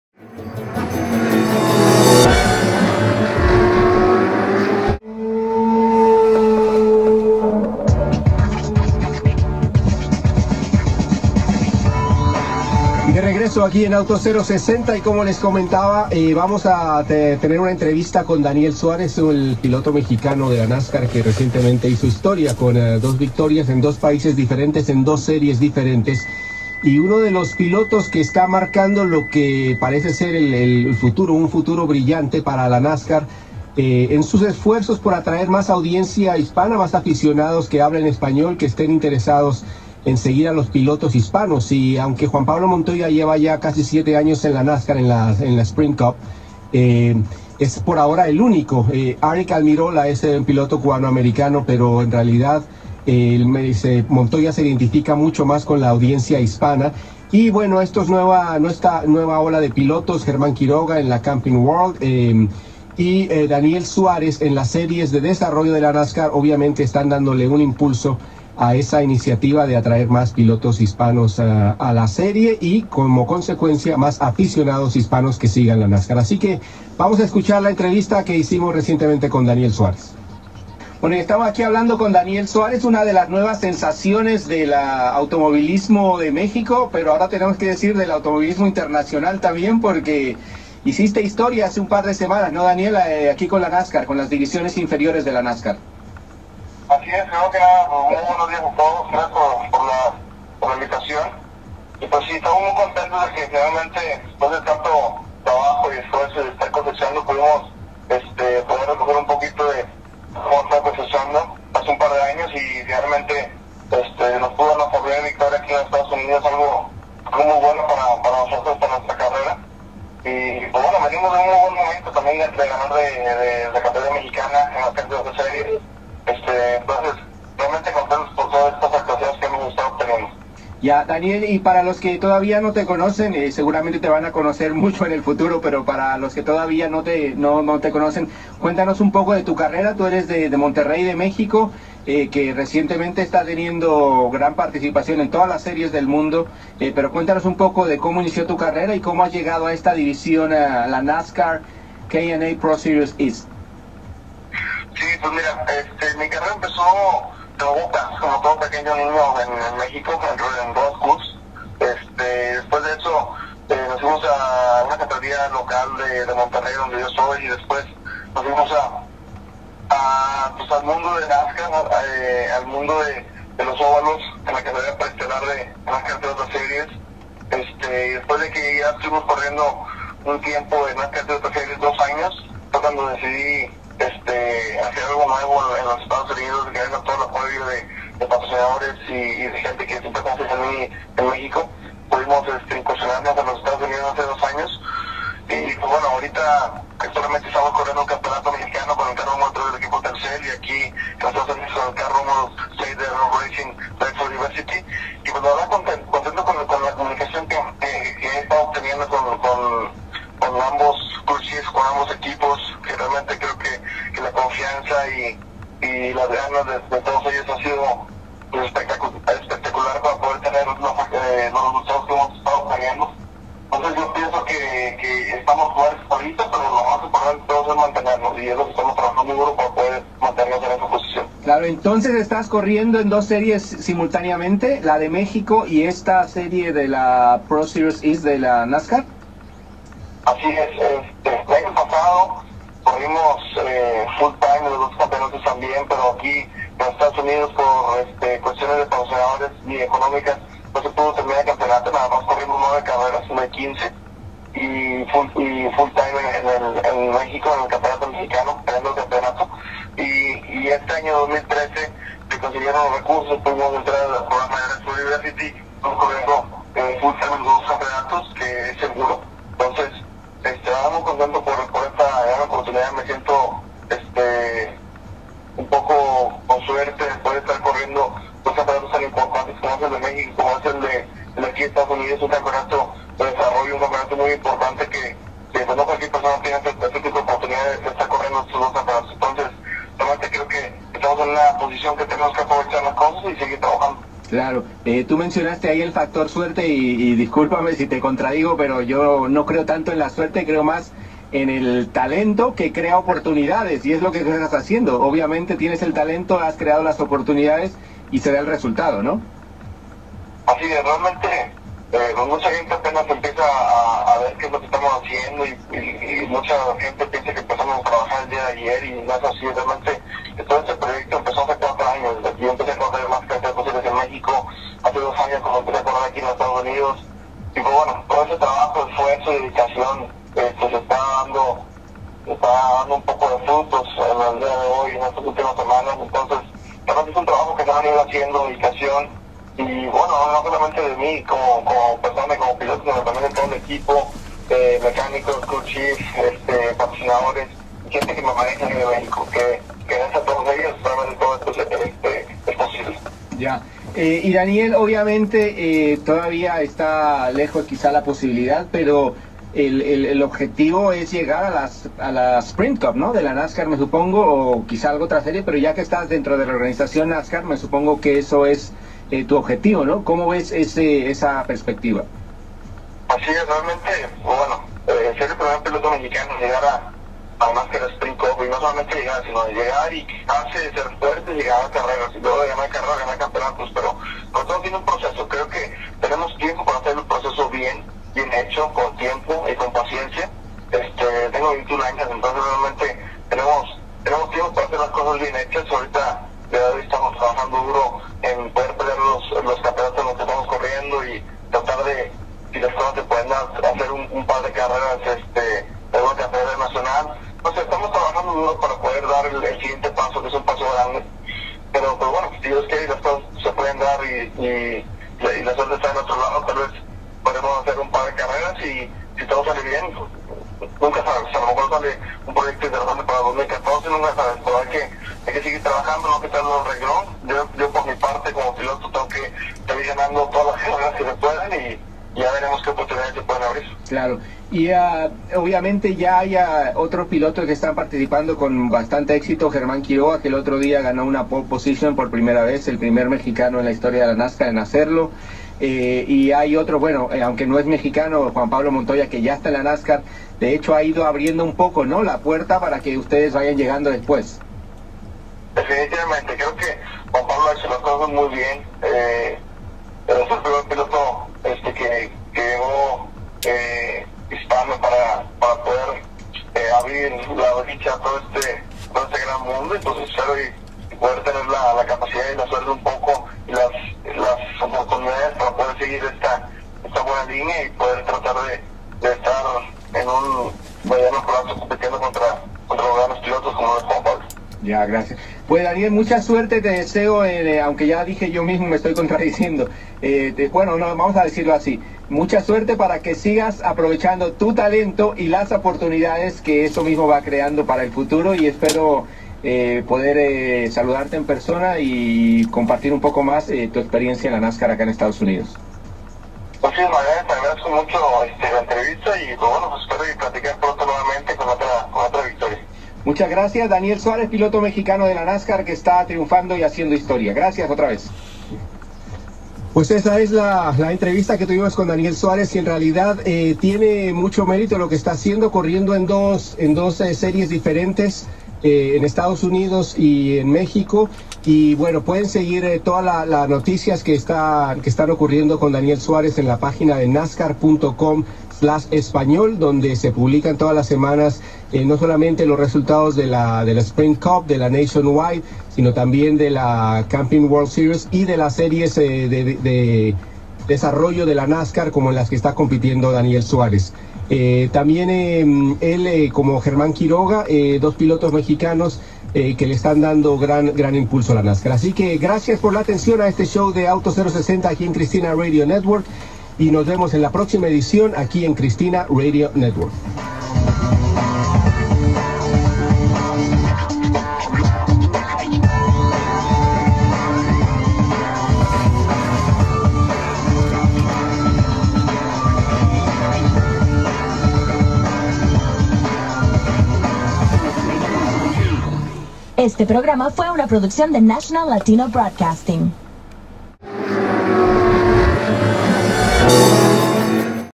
Autos 0-60 - Daniel Suárez en busca de la NASCAR Sprint Cup Series
El pasado 13 de julio, Daniel Suárez hizo historia al conseguir la bandera a cuadros de la NAPA 150 en la NASCAR K&N Pro Series East. El piloto mexicano de 21 años se impuso sobre la pista del Columbus Motor Speedway, días después de haber conquistado en México la carrera de la NASCAR Toyota Series.

El joven originario de Monterrey vive su mejor momento como piloto en las divisiones inferiores de la NASCAR, perfilándose como una de las nuevas sensaciones hispanas en el mundo del automovilismo internacional.